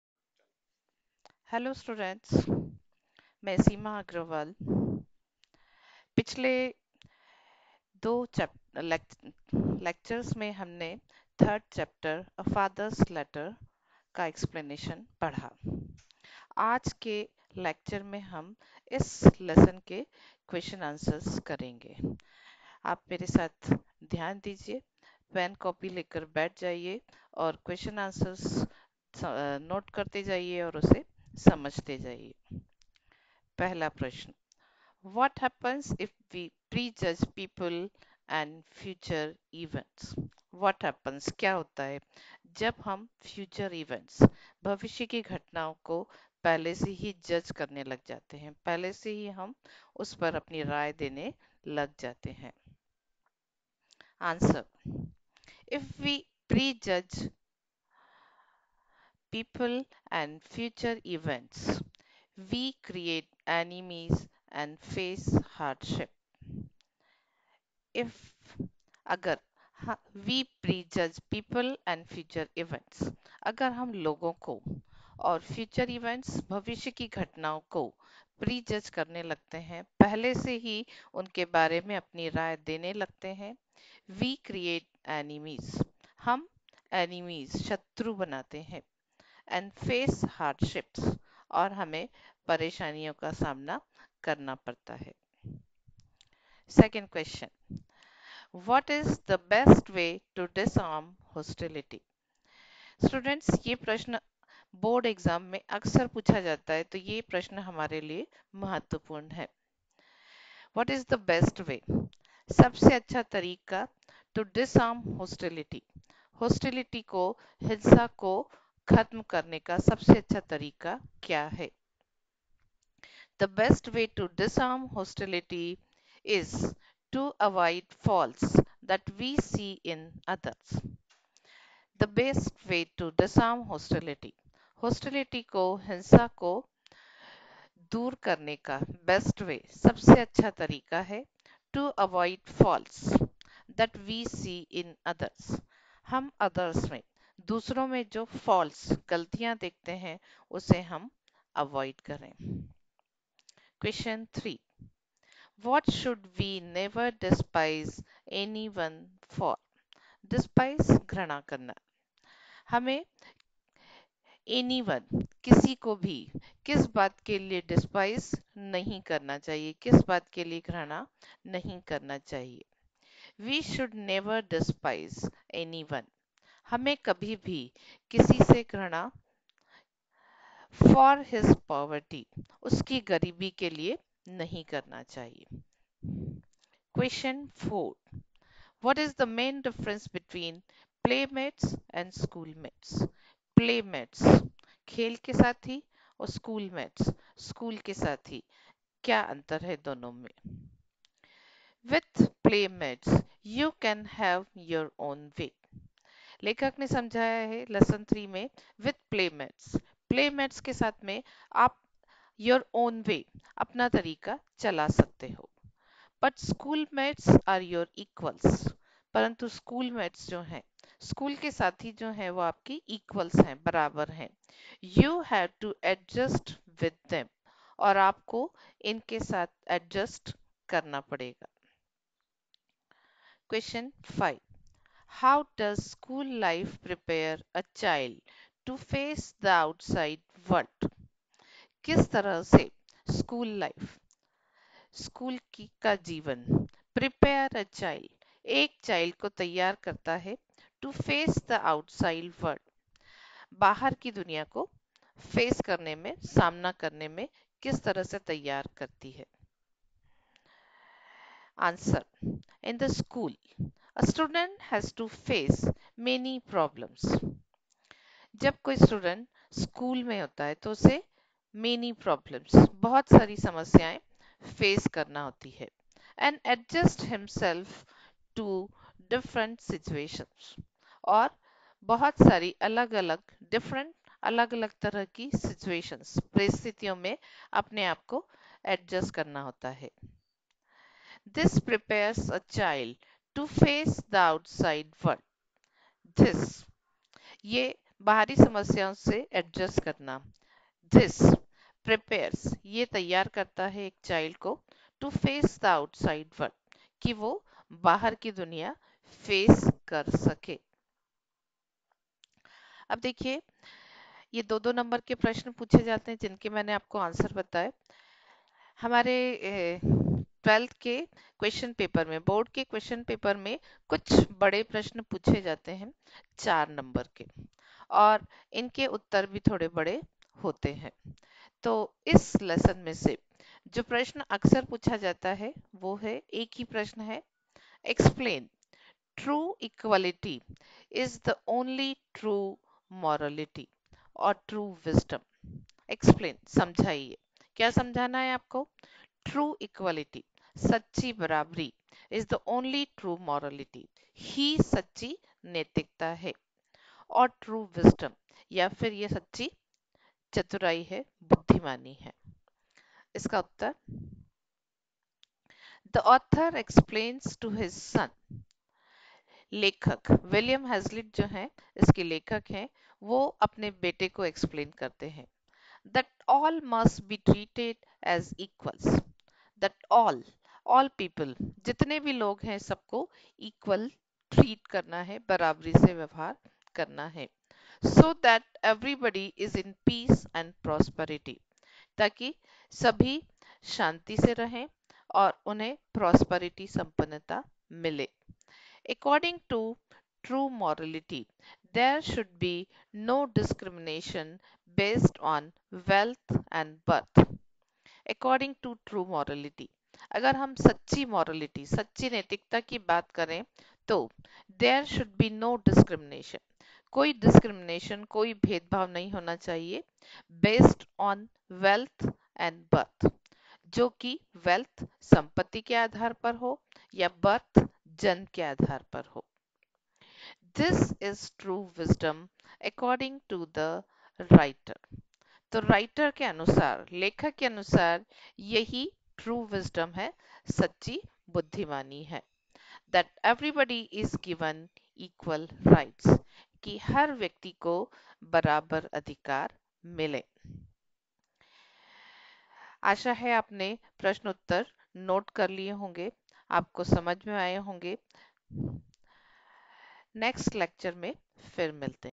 A Father’s Letter - Question and Answer
Question and Answer of A Father’s Letter, chapter 3, General English MP board.